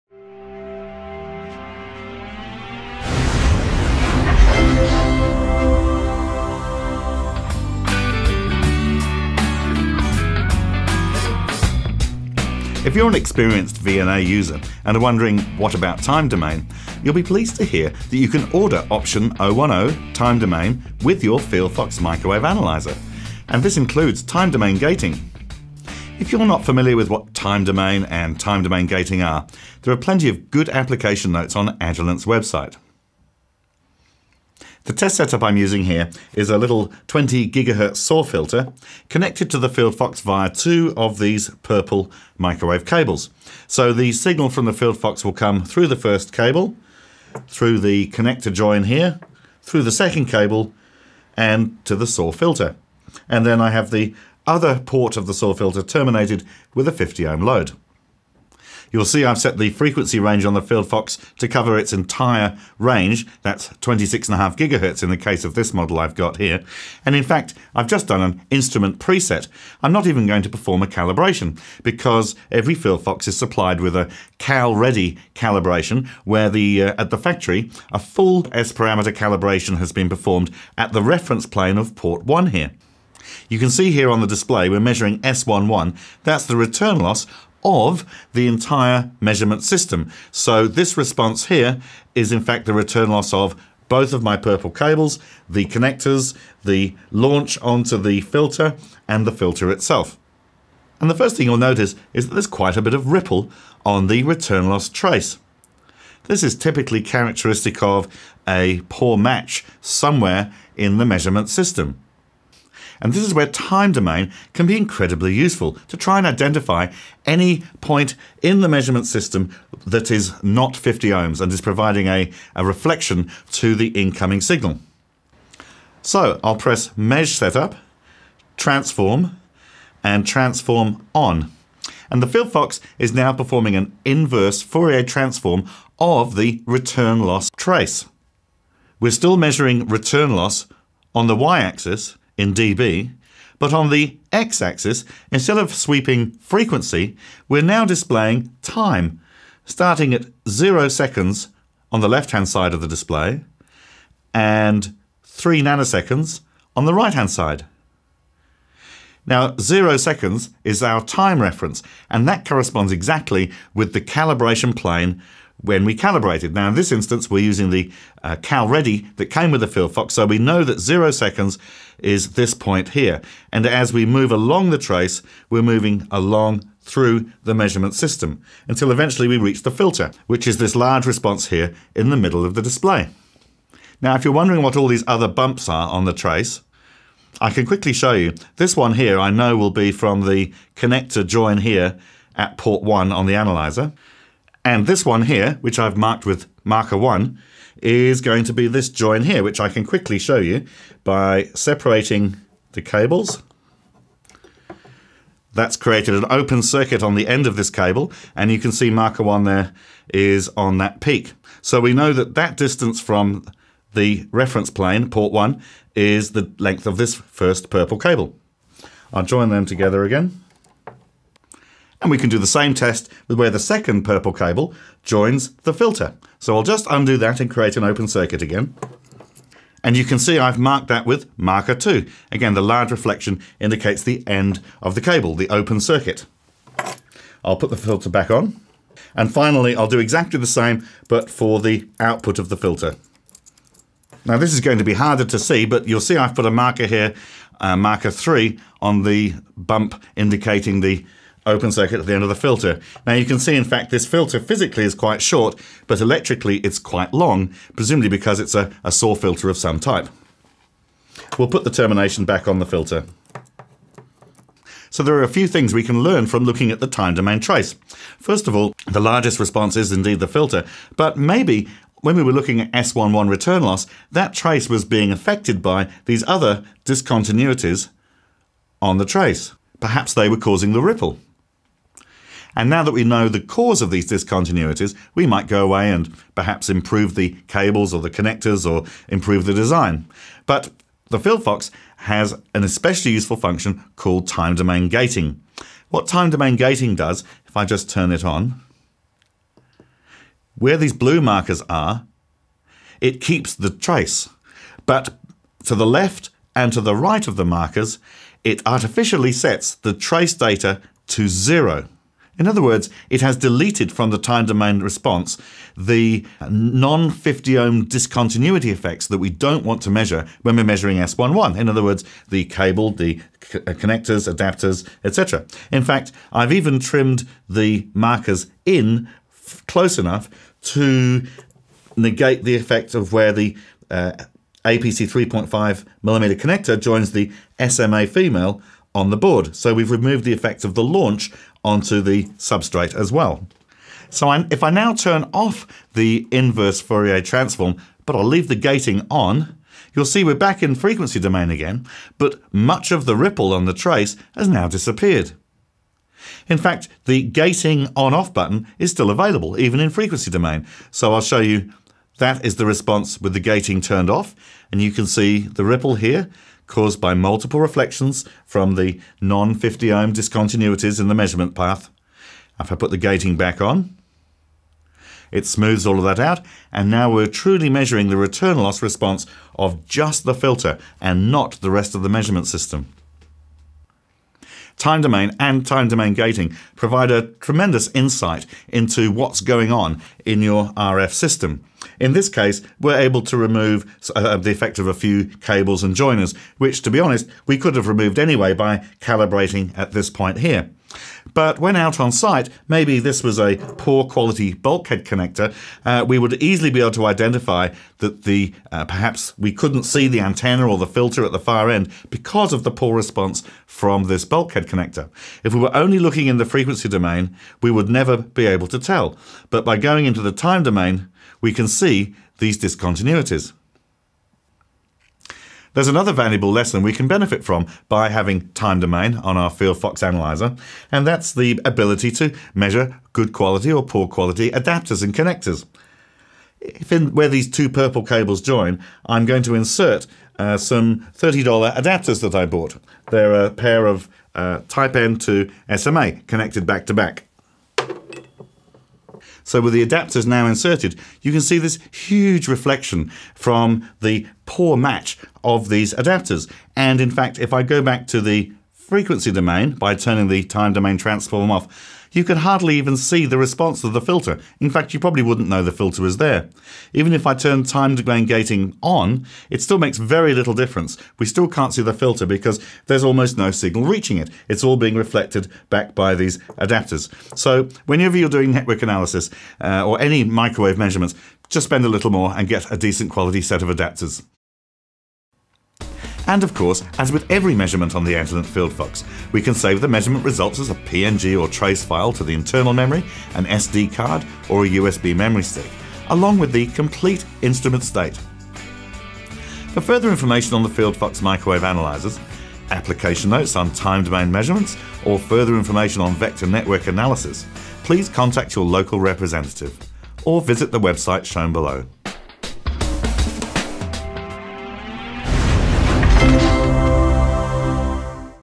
Time Domain Measurements | FieldFox Handheld Analyzers | Keysight Technologies
See how a FieldFox handheld analyzer can be used to perform time domain measurements in the field. This video demonstration (4 of 8) includes an explanation of time domain basics and time domain gating, examples of measuring the quality of cables, adapters, connectors, and identifying and removing mismatch discontinuities.